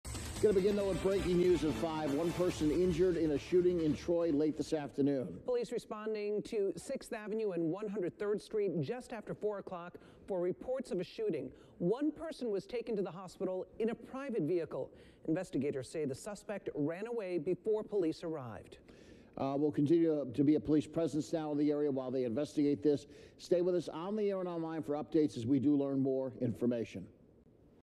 Police respond to reports of a shooting in Troy
Troy Police are responding to a report of a shooting in the area of 6th Avenue and 103rd Street. A victim was taken to a local hospital by a private vehicle.